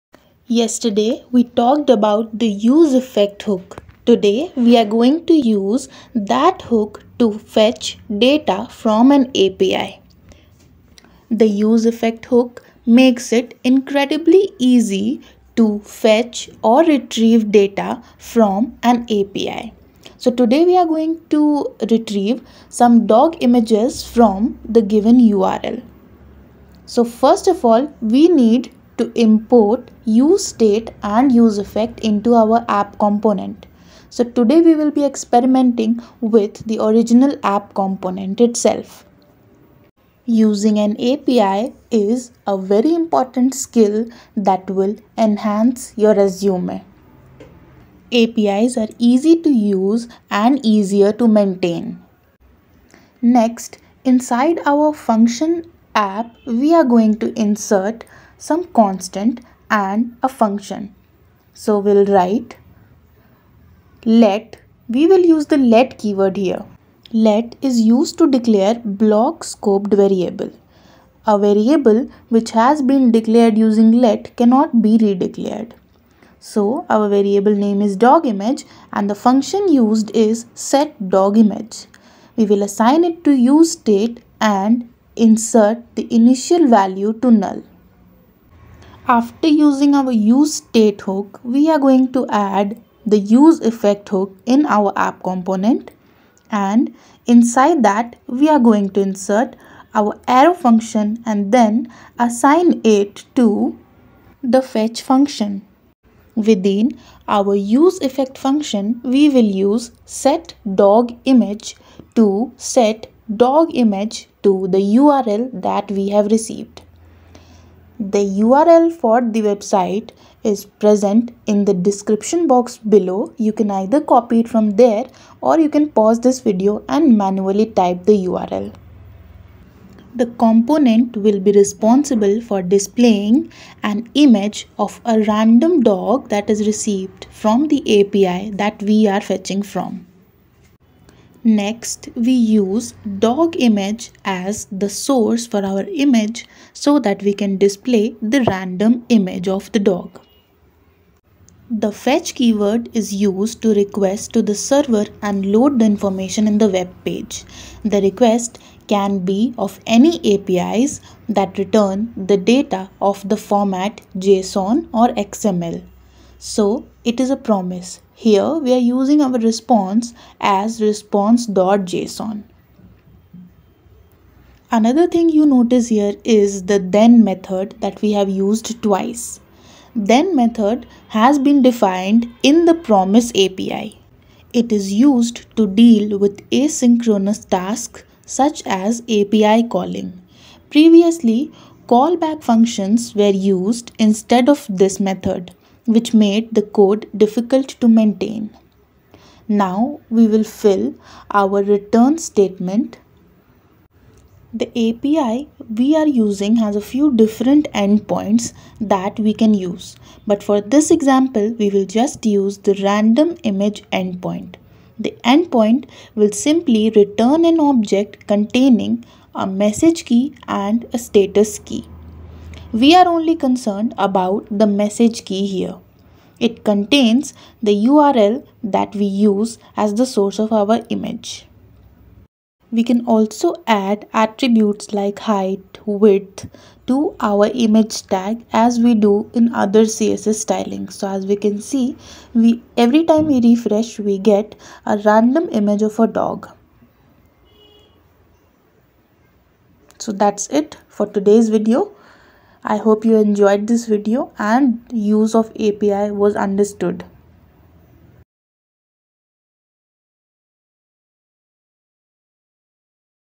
Fetch Data from API with useEffect in React JS | EP - 8
This video will quickly go over how to make use of the useEffect hook in React to retrieve data from an API.

This video assumes that you have a general understanding of how to fetch/retrieve data from an API as well as the fundamentals of React and React Hooks.

The component will be responsible for displaying an image of a random dog that is received from the API that we are fetching from. To do this, we'll need to:

Import useState and useEffect
Create our dogImage variable as well as the setDogImage function via useState
Create out useEffect function — this is where we'll perform our fetch
Within our useEffect function we'll use setDogImage to.. well... set dogImage to the image url that we received
Use dogImage as the src for our image so that we can display the random dog

The API that we're using has a few different endpoints that we can use, but for this simple example we'll just use the random image endpoint. This endpoint will simply return an object containing a message key and status key, we're only concerned about the message key in this simple example. The message key will contain the url that we'll use as the source for our image.